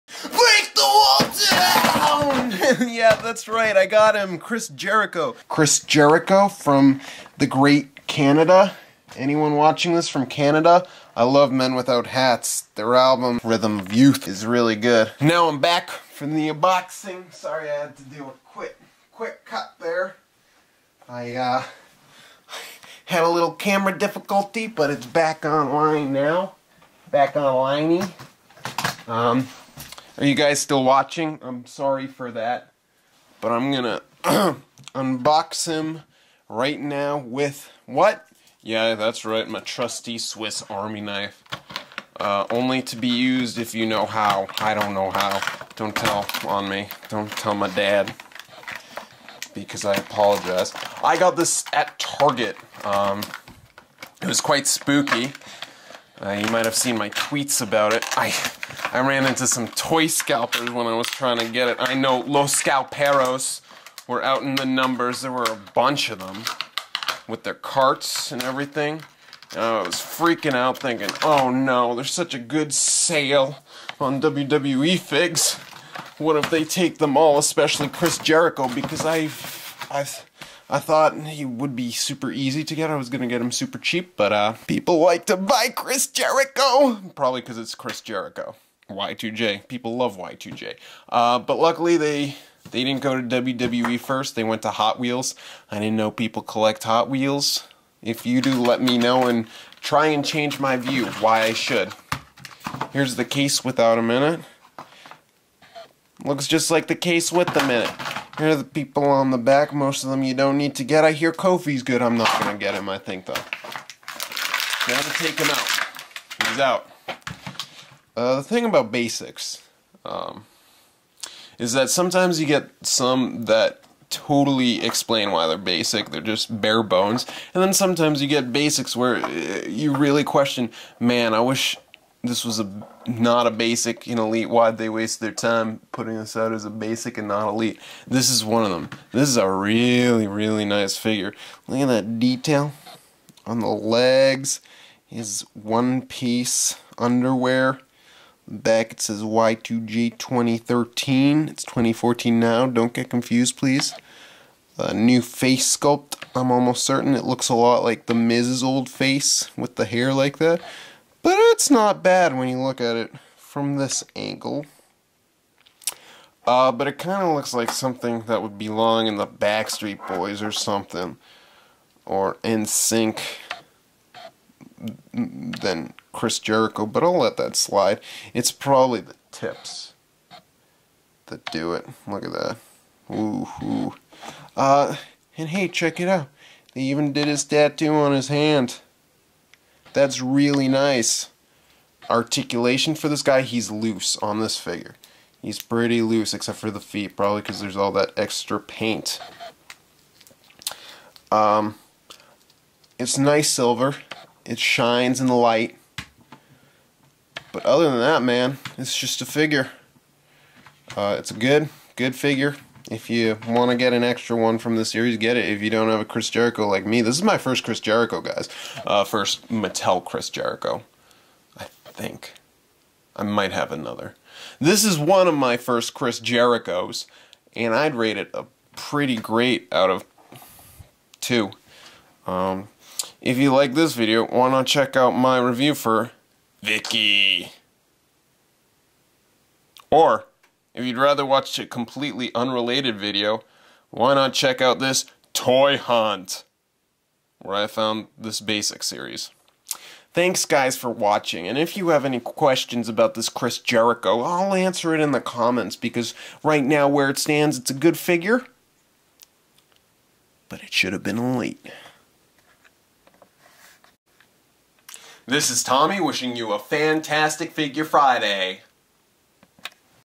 Chris Jericho WWE Mattel Basic 38 Unboxing & Review!!
Break the walls down WITH TOMMY! Cool new Chris Jericho